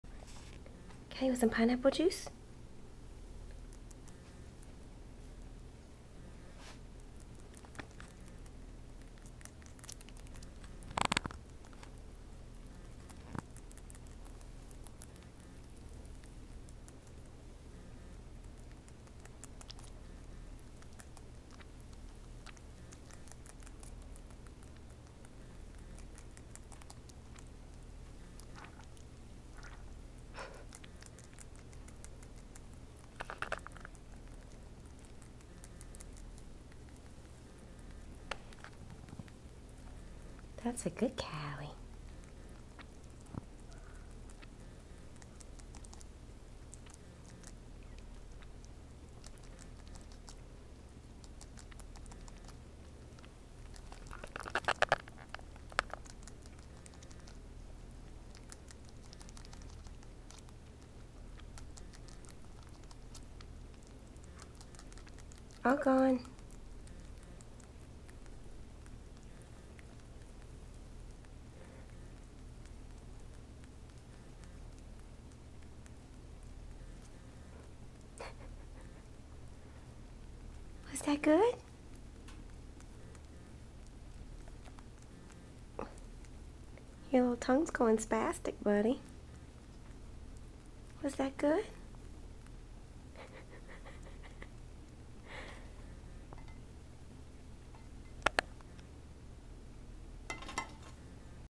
MacKenzie Bunnies - Cali's Oh So Tasty Pineapple Juice
Cali sure does love his pineapple juice! The funny part is after he's finished!

(NOTE: Pineapple juice IS safe for bunnies and is actually vet recommended (as is 100% pure canned pumpkin) for their G.I. tracts - there are enzymes in pineapple juice and pumpkin that break down the ingested fur in their tummy's)